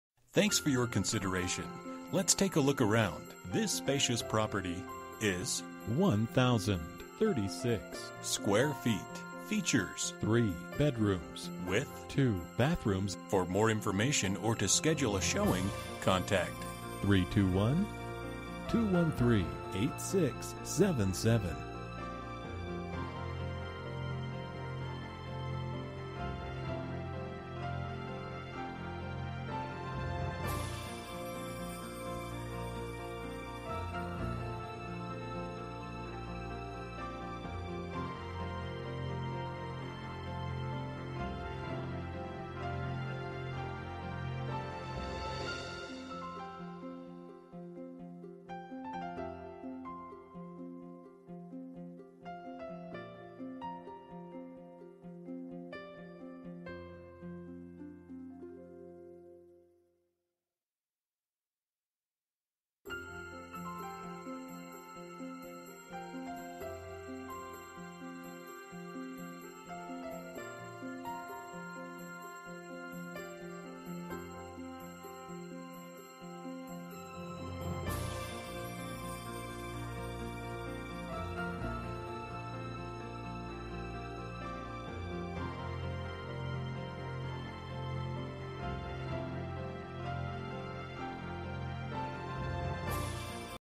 Homes for sale - 2535 Stratford Drive, Cocoa, FL 32926
The perfect place to call home! Completely remodeled to include a brand new metal roof and gutters, granite counter tops, under mount stainless steel kitchen sink, new stainless steel appliances.  Plus hard wired smoke detectors and new doors throughout as well as a new slider in DR. HVAC, tile floors, all new plumbing in and under the house to the road, garage door about 5 years ago.  Also includes updated electrical panel, outlets and switches. Nothing has been overlooked here.

Bedrooms: 3
Bathrooms: 2
Square Feet: 1036
Price: $129,900

07/28/2018 03:37:03 pm

Last modified: